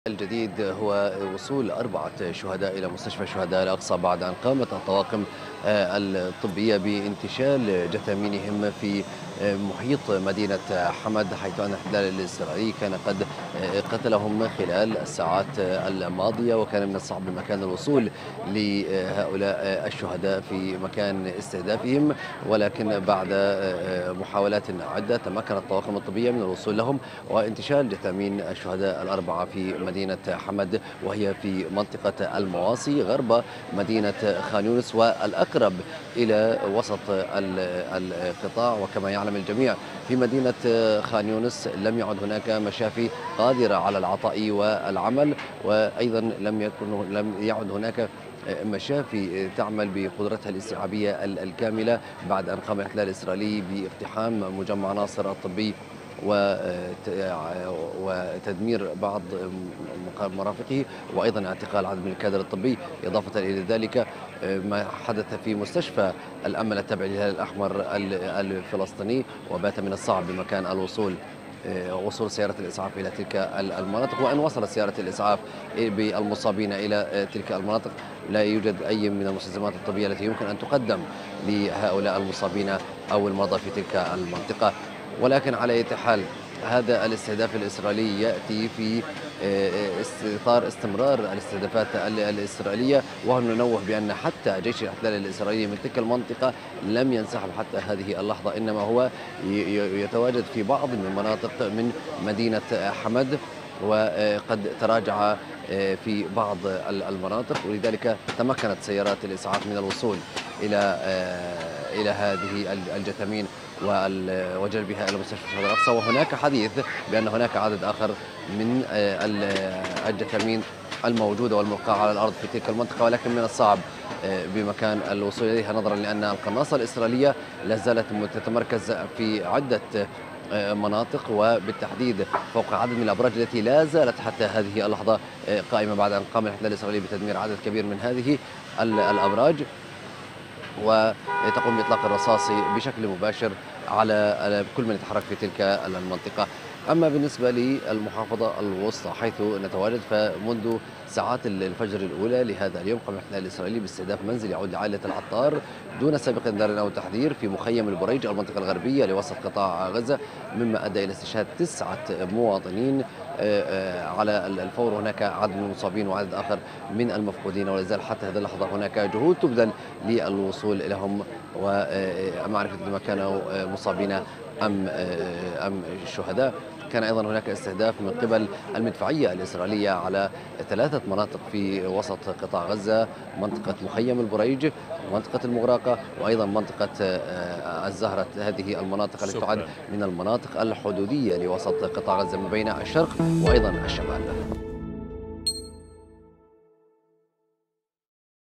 انتشال جثامين 4 شهداء من مدينة حمد في خان يونس
مستمرون في تغطيتنا من قناة_الغد للعدوان الإسرائيلي على قطاع غزة، حيث يواصل الاحتلال الإسرائيلي خلال شهر رمضان غاراته العنيفة على مناطق متفرقة شمالي وجنوبي مدينة غزة.. المزيد من التفاصيل يتحدث مراسل الغد من دير البلح محمد أبو ناموس مع حسين حسني، عبر الفقرة الإخبارية.